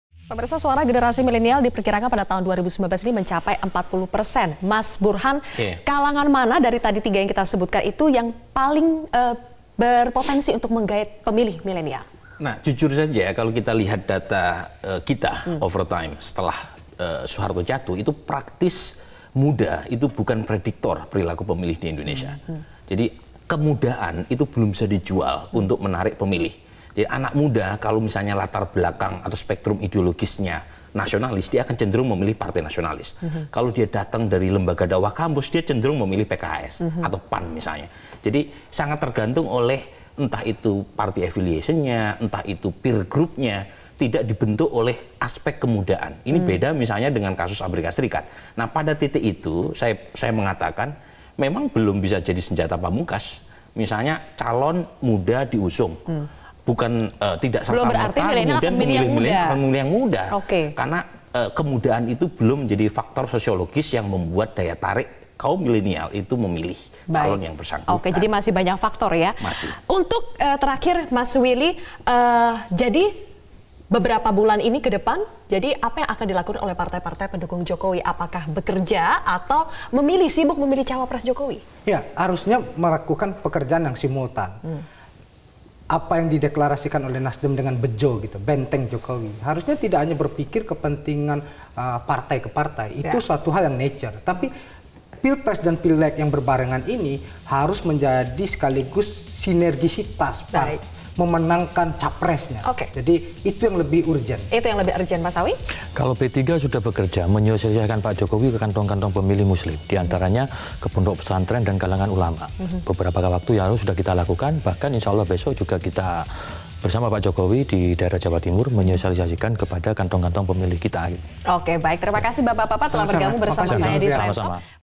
Sosok Anak Muda Tidak Menjamin Gaet Pemilih Milenial
pilpres 2019
Pilpres 2019
29 Maret 2018 01:27 wib

Sosok agamis, militer atau profesional dinilai tidak menjamin bisa menggaet 40% suara pemilih milenial di Pilpres 2019. Kaum milenial atau anak muda akan memilih capres atau cawapresnya berdasarkan ideologi yang diikuti.